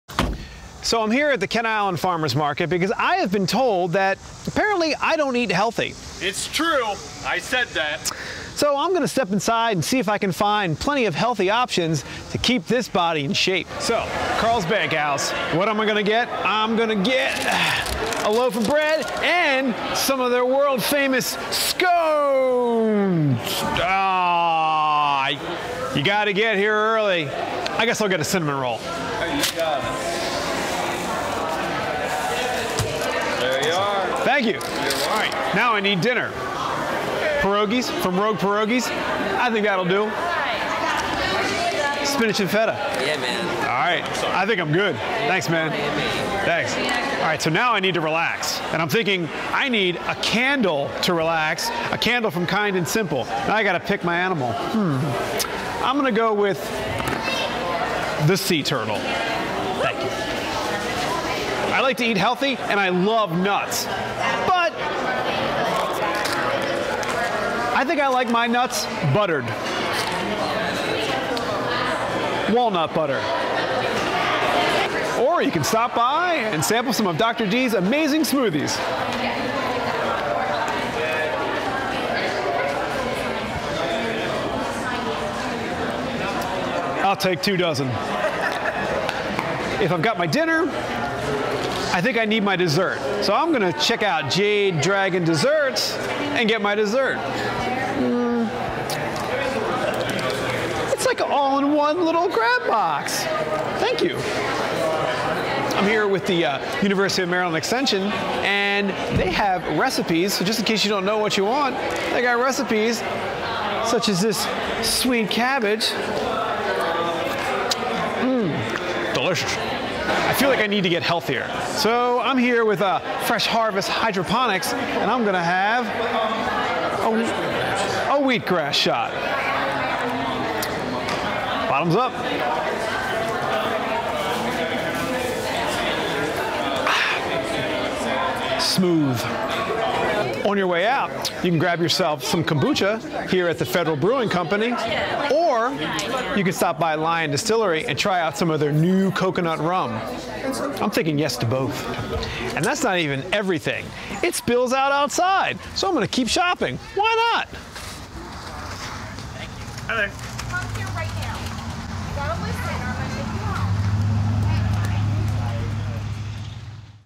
Kent Island Farmers' Market
Chris discovers all the great food at the Kent Island Farmers Market.